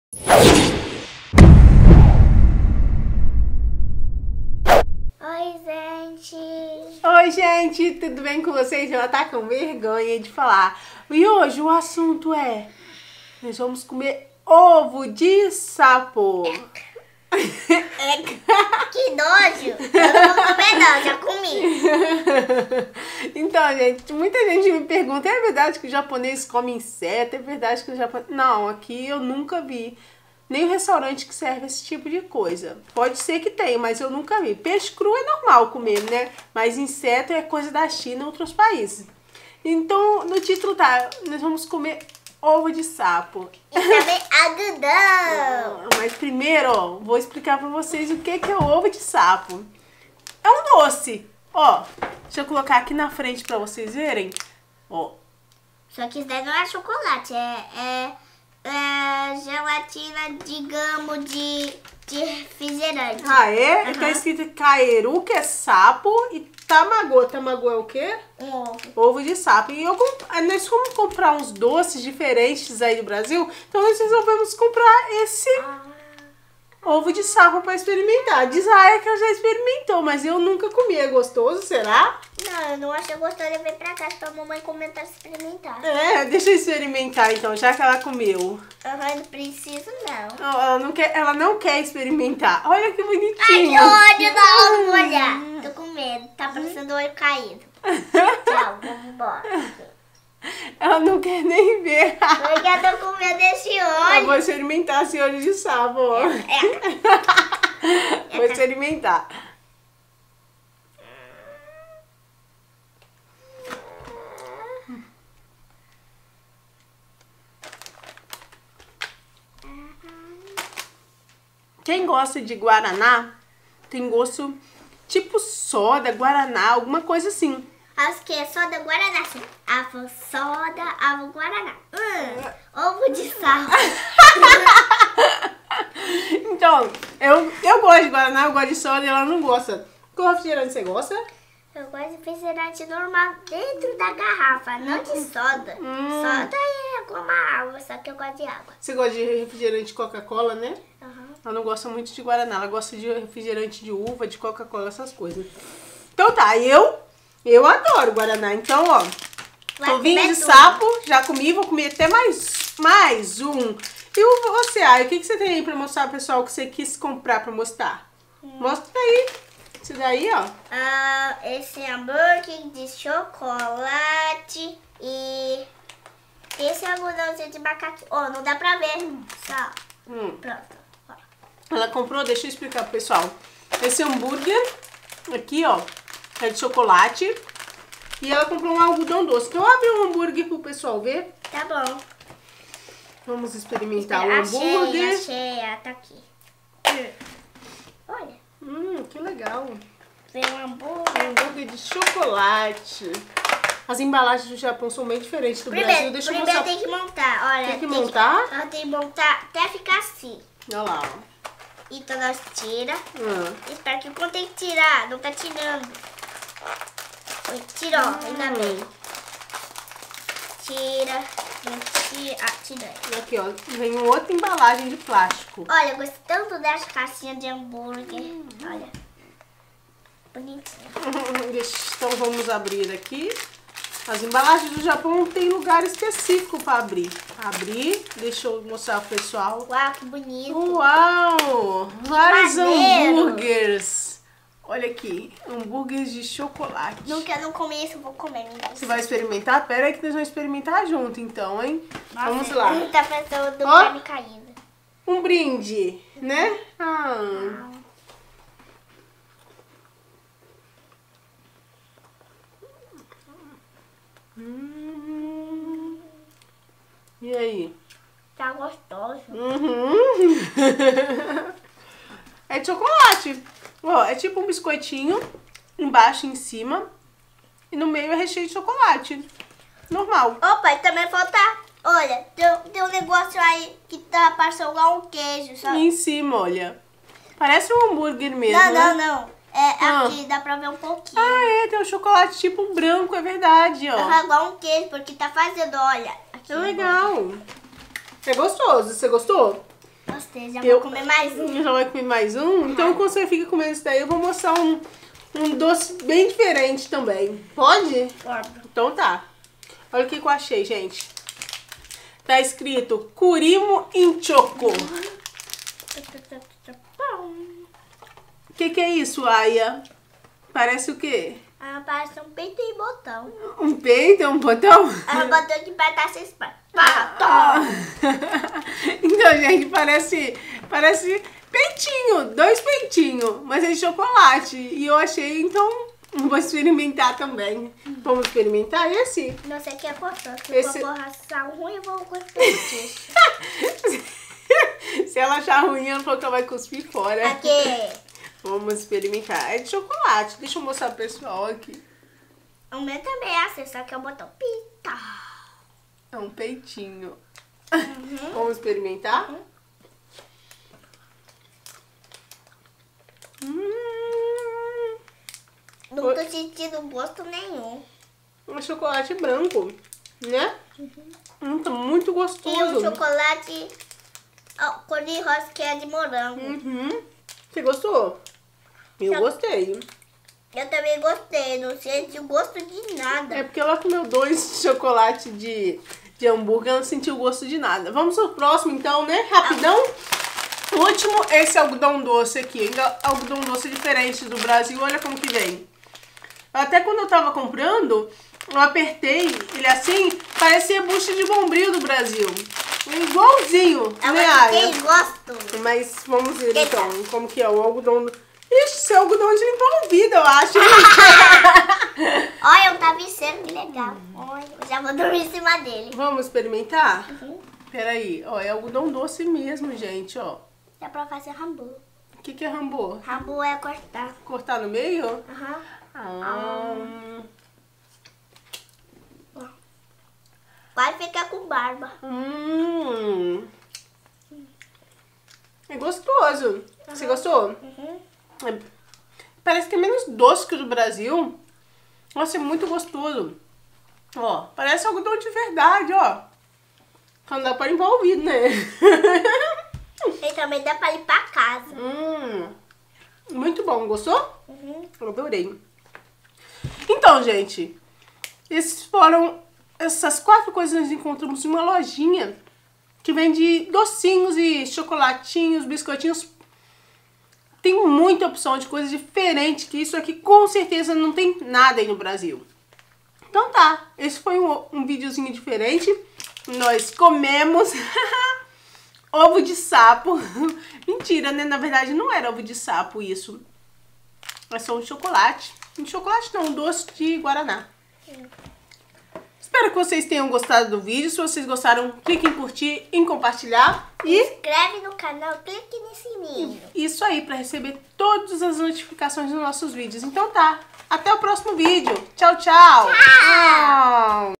COMENDO OVO DE SAPO
Experimentando alguns doces diferentes.